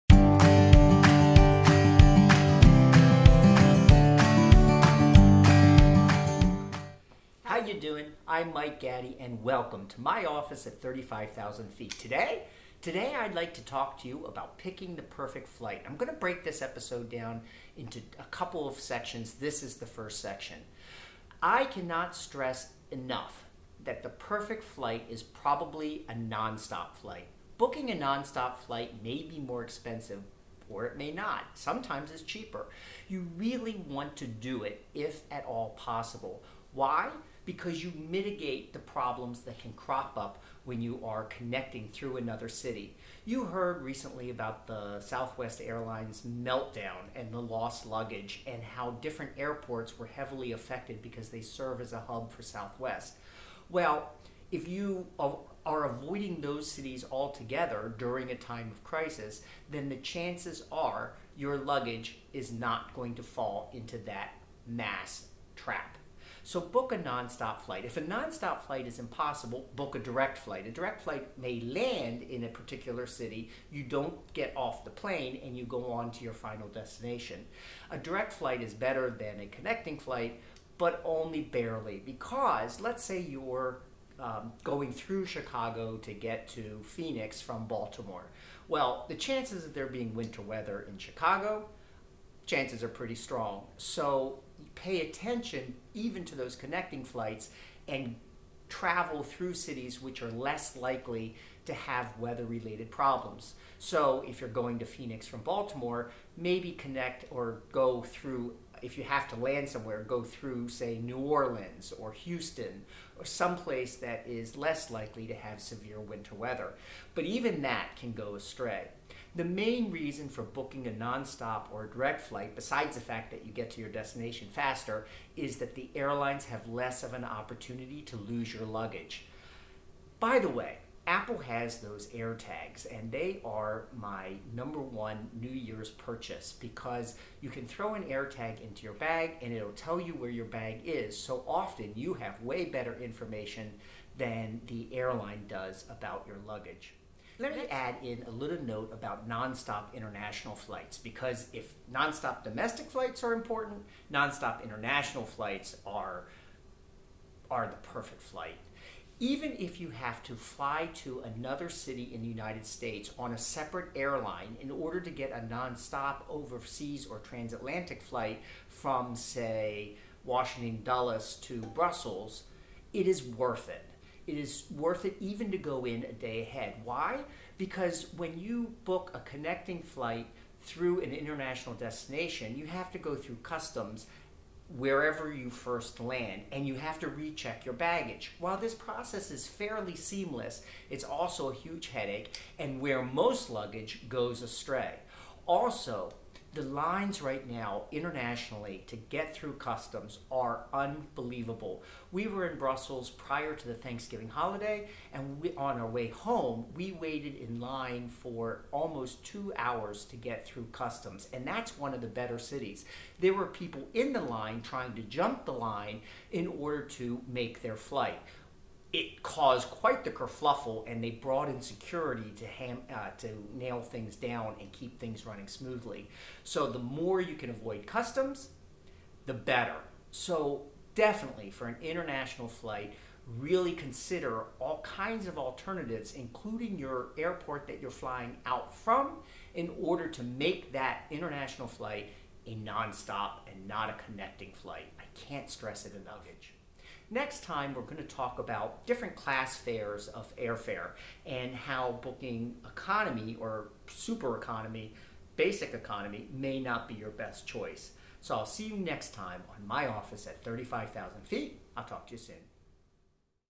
Picking the Perfect Flight
Join host Mike Gatty for this 4 minute quick tour of how to pick the perfect international or US Domestic flight for your next trip.